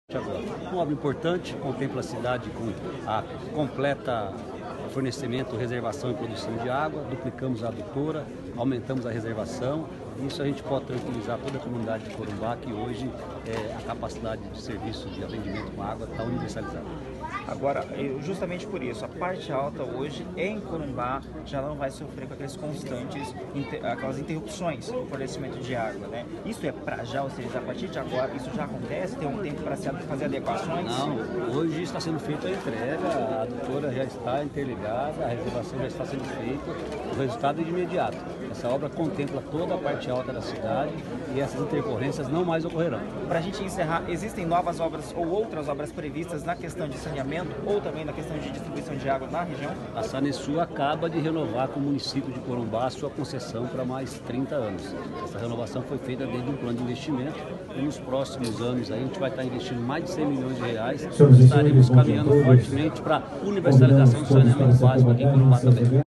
Governo do Estado entrega obras e leva mais saneamento para Corumbá
Dia importante para Corumbá, quando o Governo do Estado, por meio da Sanesul, entrega obras e leva mais saneamento para a cidade.
"Vivemos um momento histórico em Corumbá“, comentou o diretor-presidente Walter Carneiro Junior.
São obras de ampliação do sistema de abastecimento de água e de esgotamento sanitário. Feitas com recurso federal e próprio, no valor de R$ 60,4 milhões.